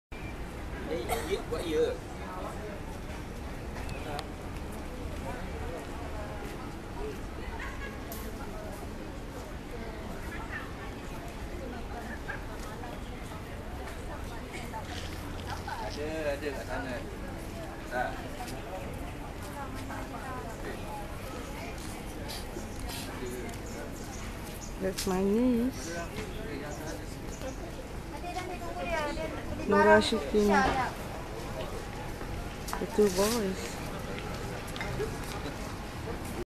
Family outing to Sultan Mosque in Singapore
Visiting one of the landmark building in SINGAPORE the Sultan Mosque.